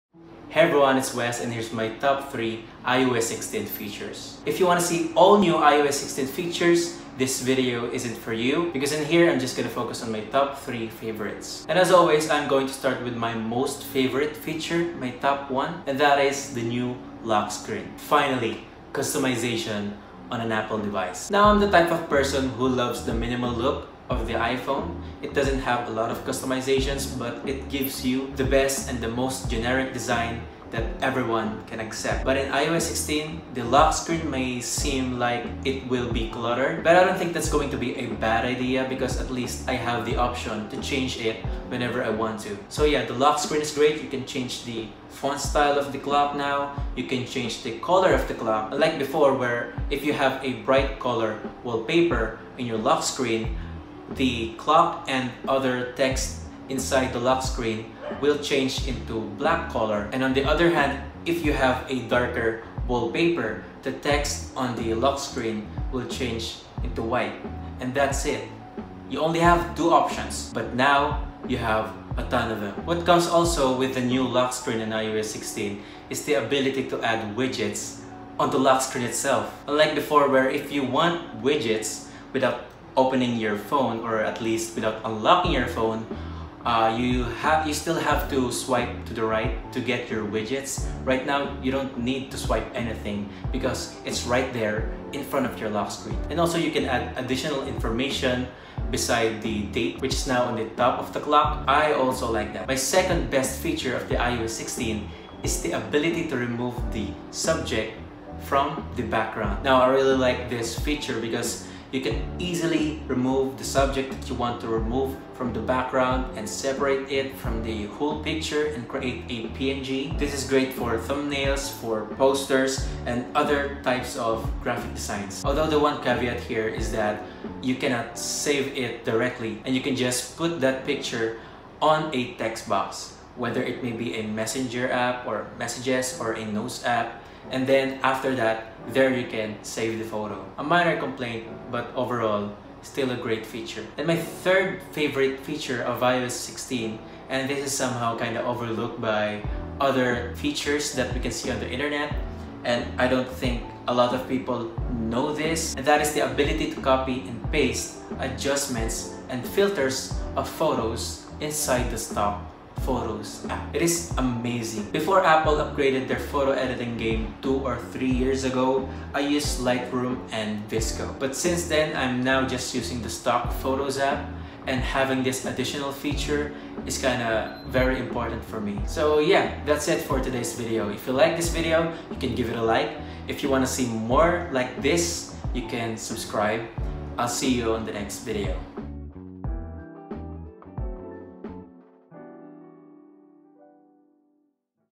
Favorite iOS 16 Features You May Not Know!
iOS 16 has been released by Apple in beta at 2022 WWDC, but based on what I see across the internet, here's my top 3 favorite features of it!

Listen with me on Spotify!